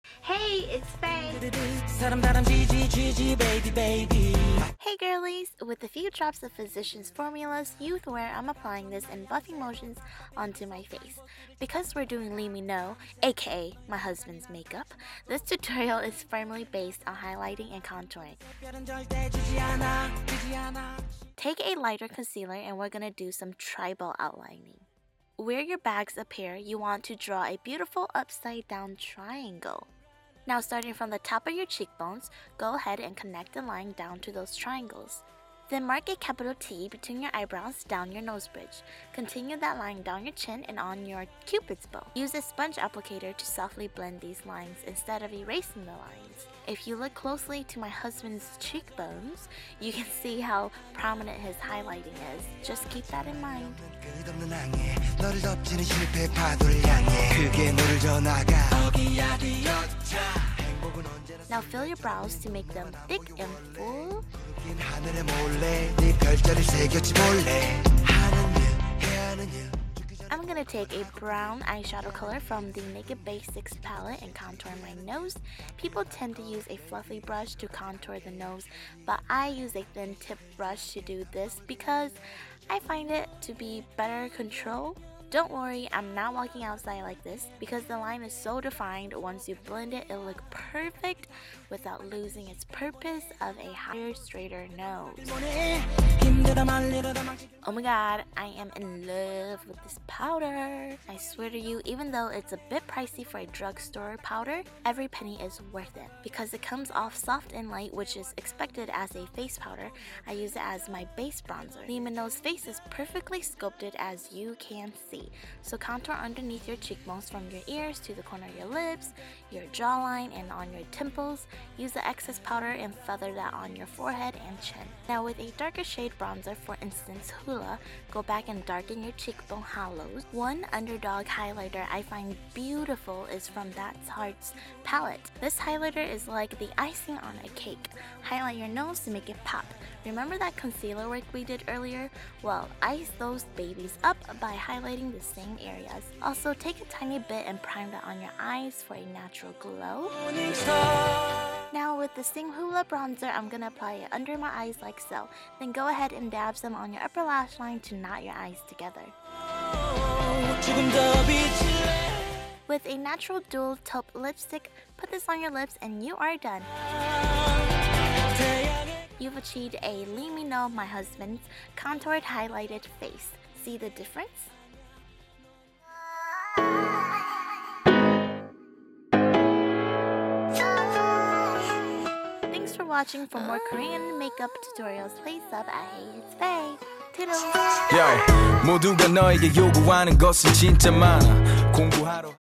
Lee Min Ho 이민호 Makeup Tutorial (Contour & Highlight)
Hi girlies! So as you may have realized, I'm married! Meet my husband! :')
Just to make clear, I have a few husbands lol!
Don't judge me! Haha

This is how to achieve his bronzed contour and highlight!

ღ L E T S T A L K ღ